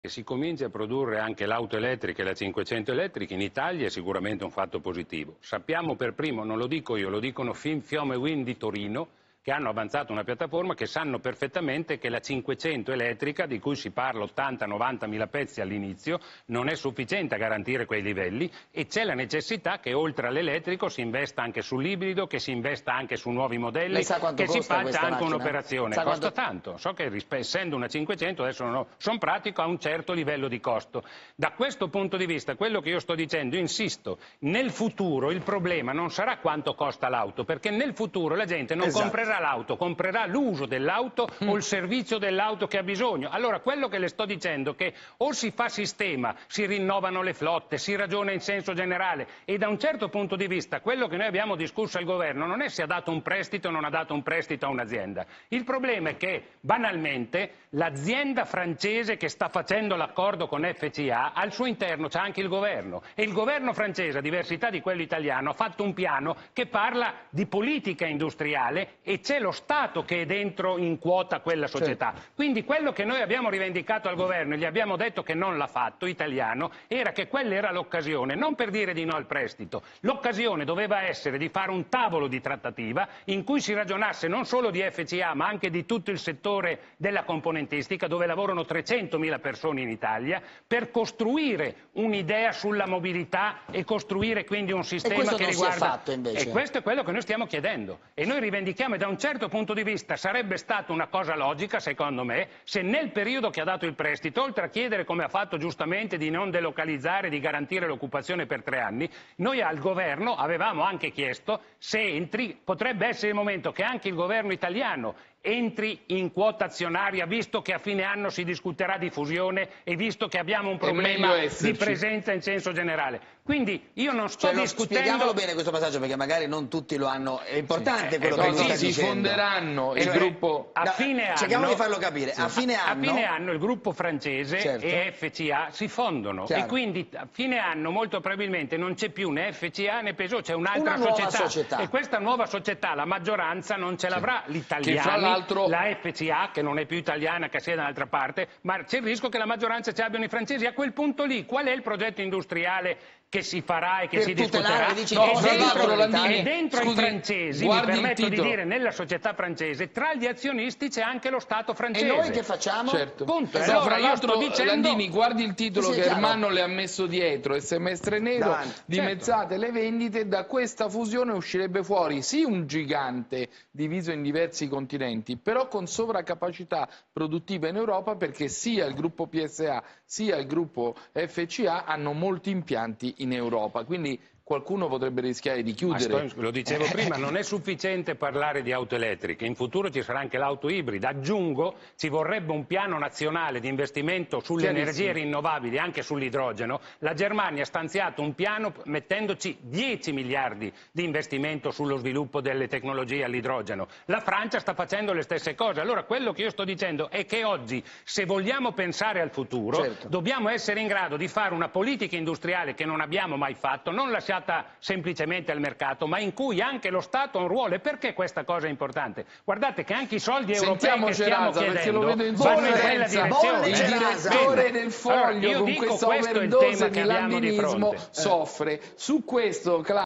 Automotive, la proposta di Maurizio Landini: "Lo Stato entri nella fusione FCA/PSA"
Per Maurizio Landini il settore automotive è cruciale per l'economia italiana: "Chiediamo un piano di rilancio di tutto il settore, lo Stato entri nella fusione FCA/PSA"